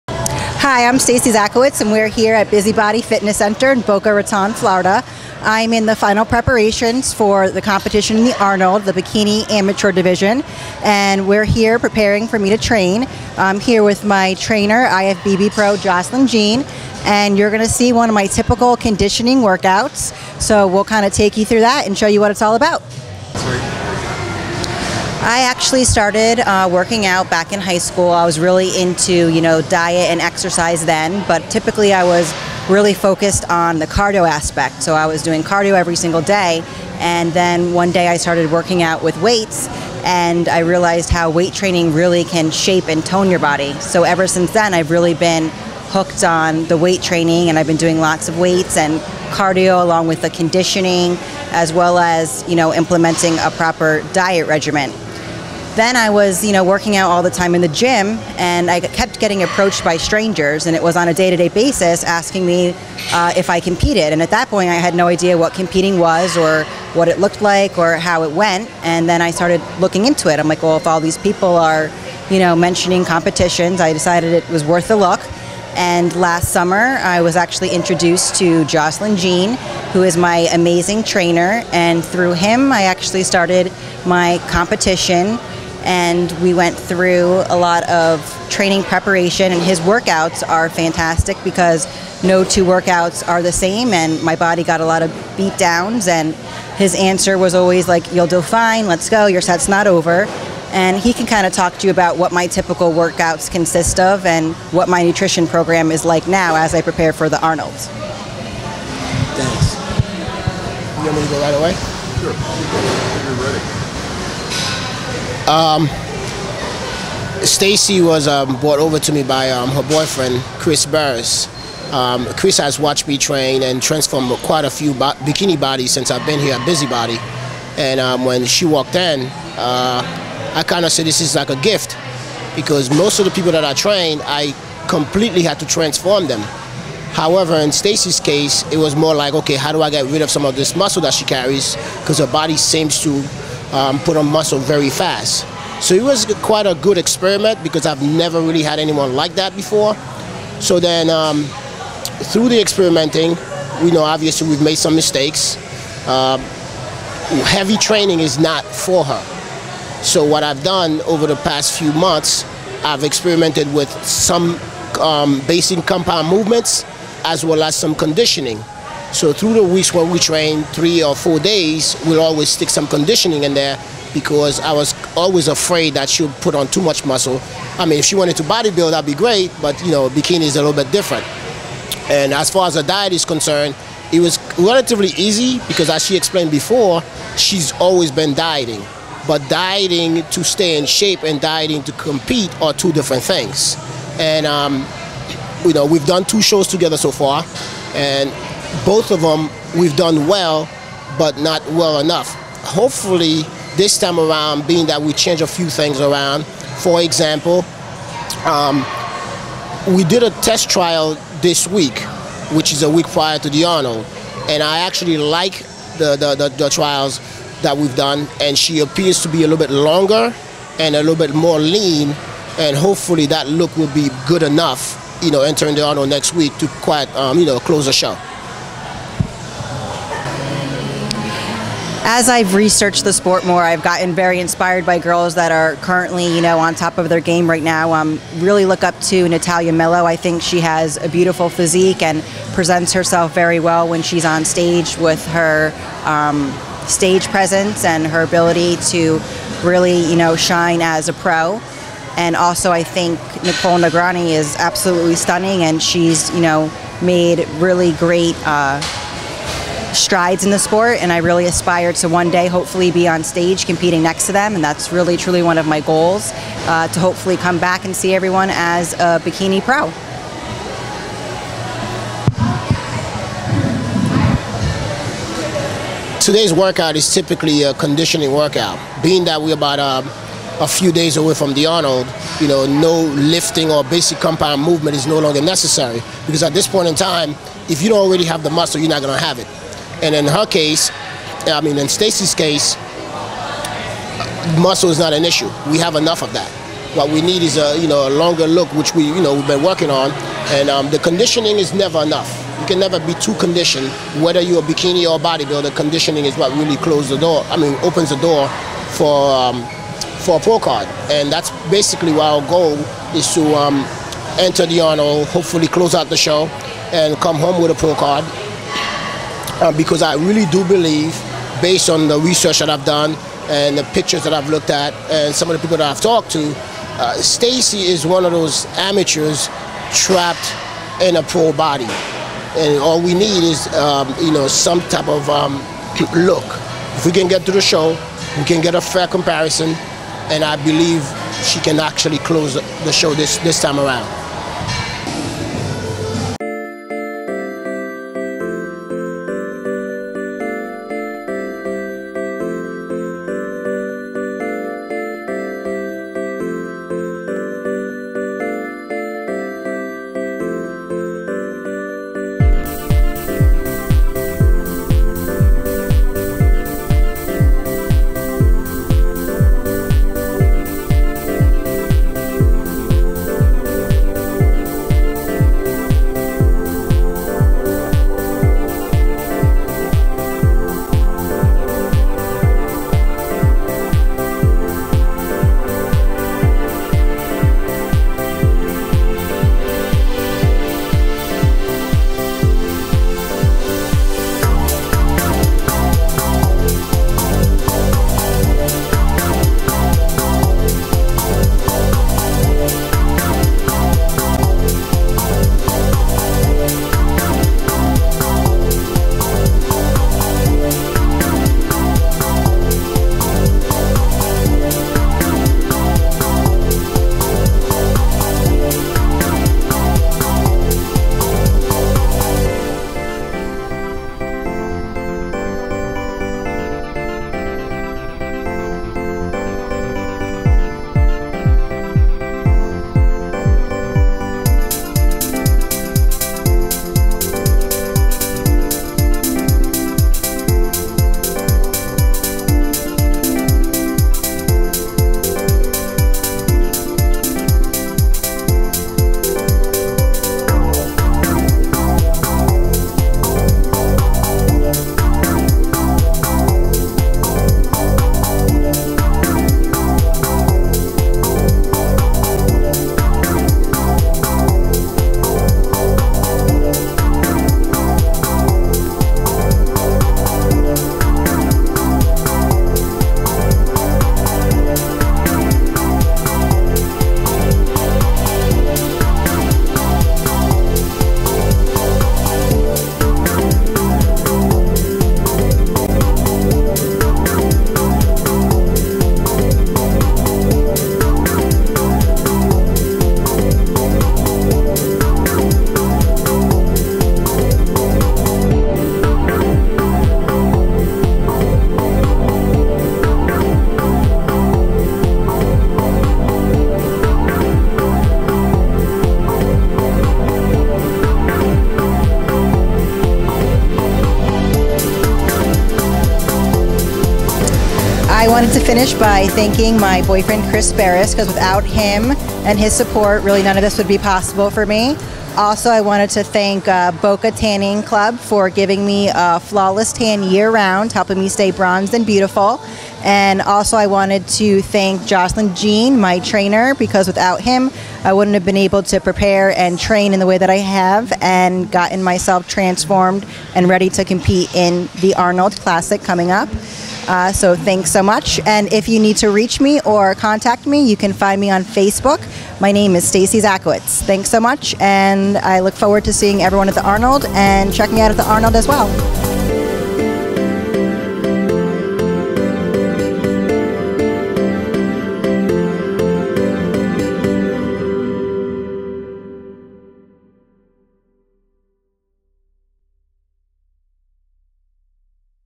Stacey Szatkiewicz Preps for Bikini with Jocelyn Jean
School teacher Stacey Szatkiewicz is striving to do well in her on studies as an aspiring NPC bikini competitor at the 2012 Arnold Classic. IFBB Professional Bodybuilder Jocelyn Jean is her trainer and they show us a sample of one of her toning and conditioning workouts six days out from Columbus.